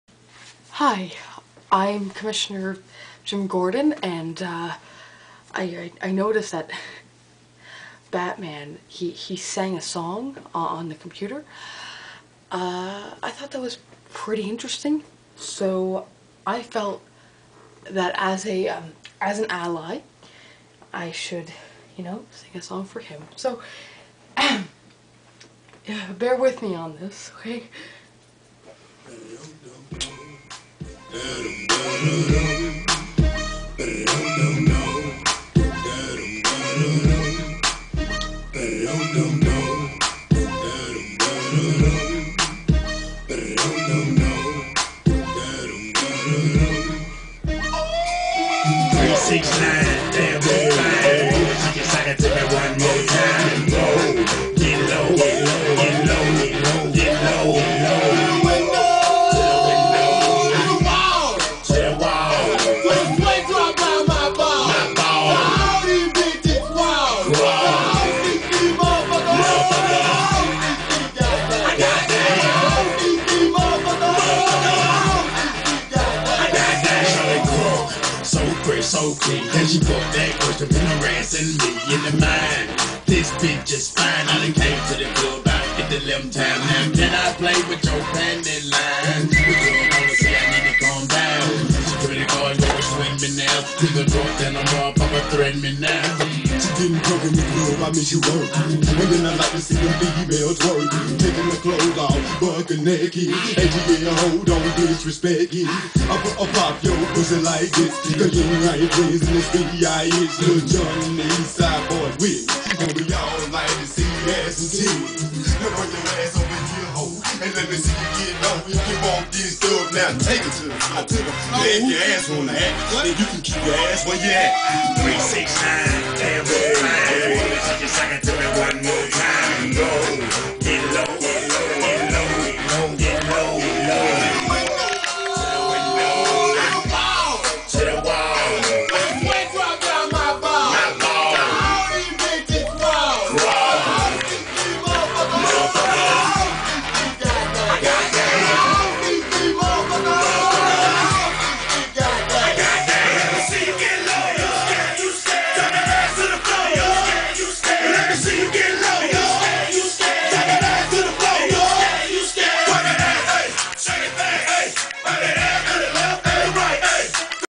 Jim Gordon Sings
Jim was feeling frisky. Wanted to show himself off to Bruce.

I love you Spinni-Sama and Jay! THIS ONE IS FOR YOU!